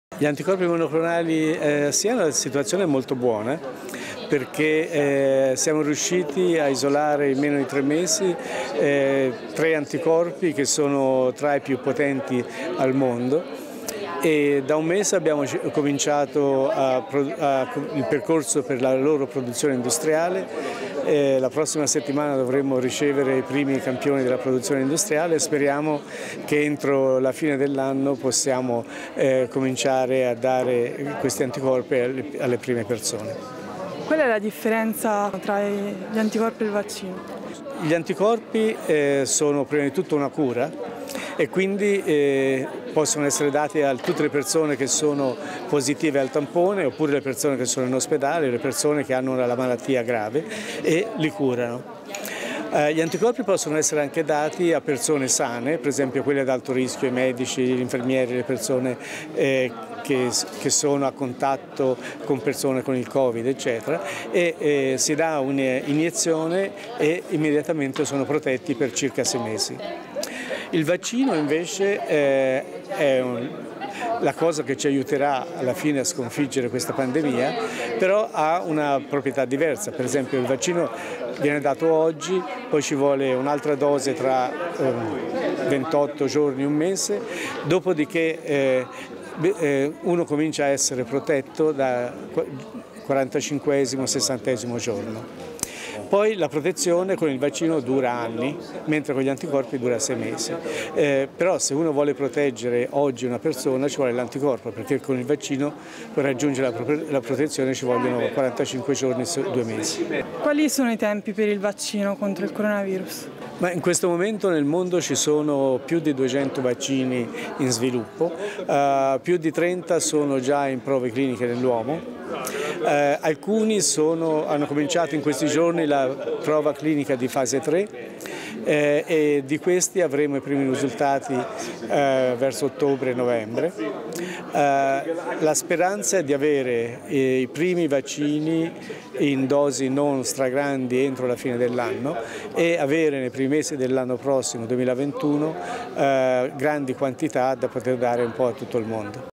Rappuoli a La Terrazza di San Casciano
Cure anti Covid, il punto di Rino Rappuoli. Rappuoli, chief scientist di Gsk Vaccines a Siena, ospite a "La Terrazza" a San Casciano dei Bagni per il talk dal titolo “Dai vaccini agli anticorpi, la nuova frontiera dei virus”, parla di di vaccini e anticorpi contro il Coronavirus (immagini di Nti)